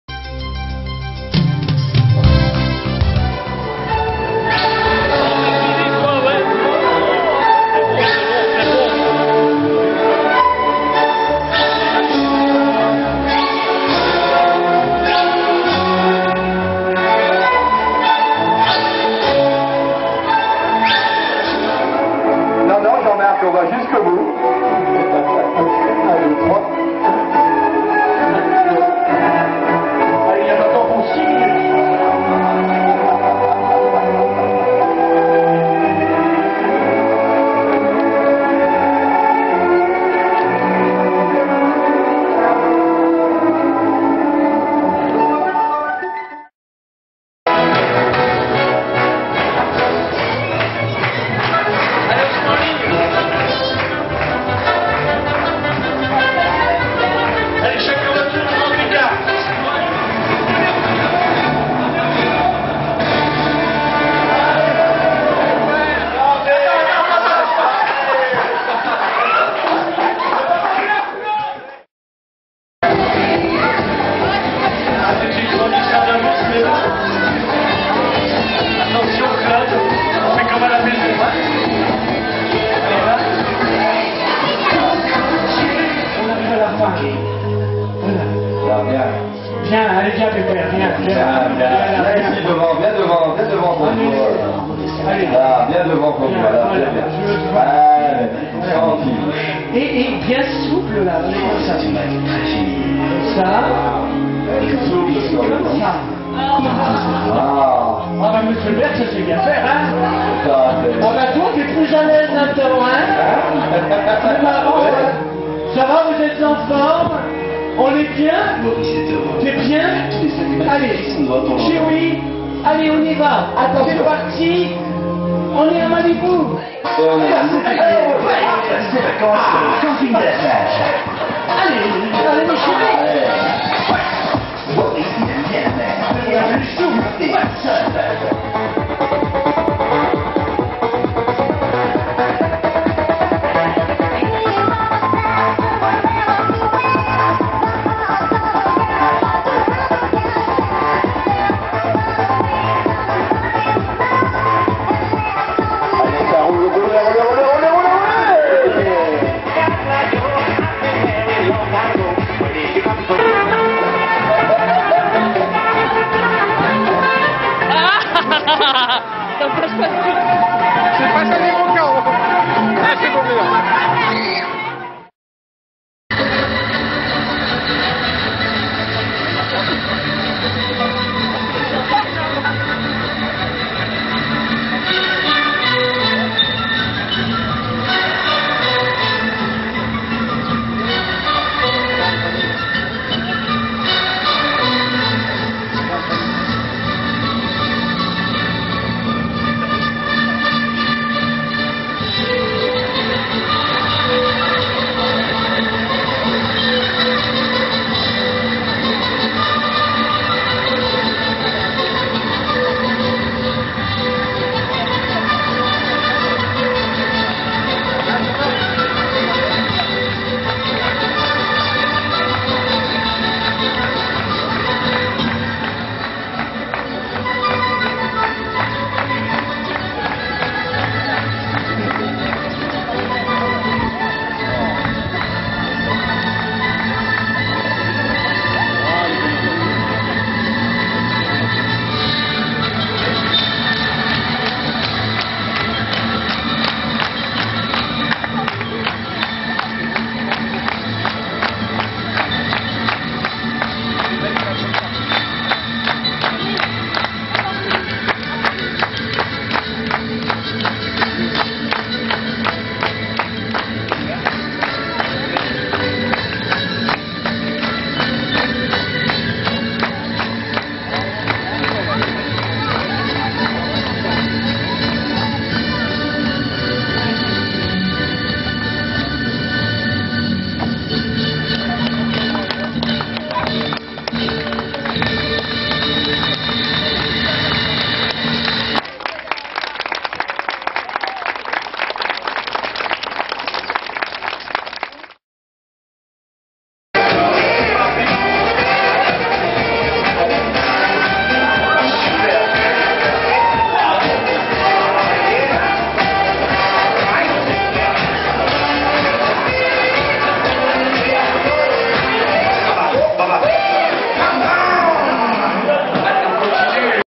ADINFER - Fête de la Pentecote 2006
Quelques images prisent sur le vif lors de la fête de la Pentecôte 2006 à Adinfer